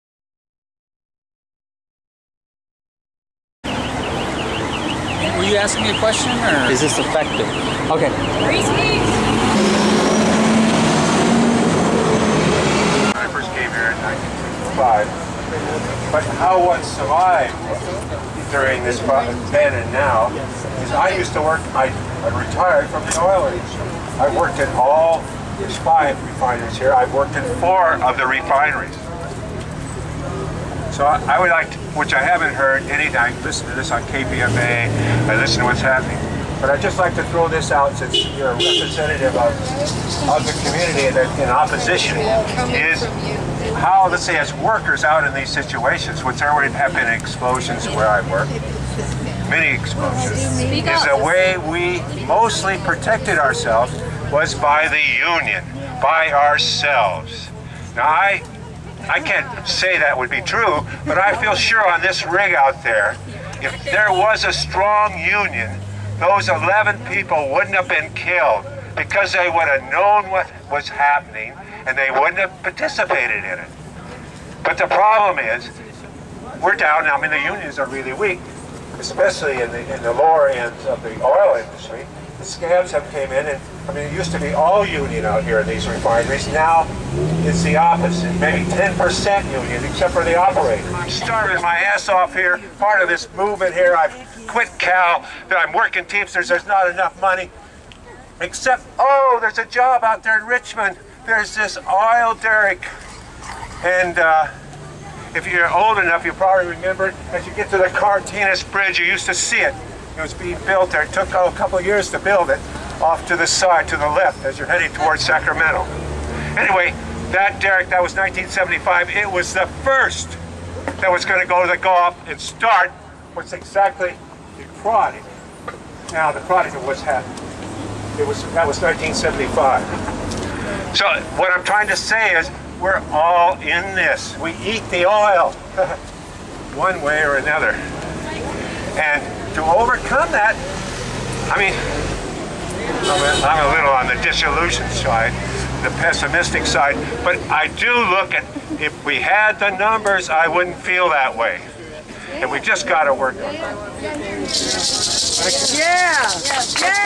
UC-BP: Protest Outside BP Helios Project
Footage outside construction site for BP's Helios project.  UC Berkeley Police stop by to check on their toxic assets. A $500Mil project, it is the largest corporate deal made in conjunction with any university.  The name is a misnomer given that the Helios project has nothing to do with solar energy.  Rather it will be a research facility for more petroleum exploits.  There is also a secondary goal of growing bio-engineered switch-grass in South America, cutting down more trees to grow the bio-engineered grass.  BP will control all research, treating professors and students as BP employees.

Rising Tide Earth First Loop Current Oil Spill Gulf Coast Blow Out Preventer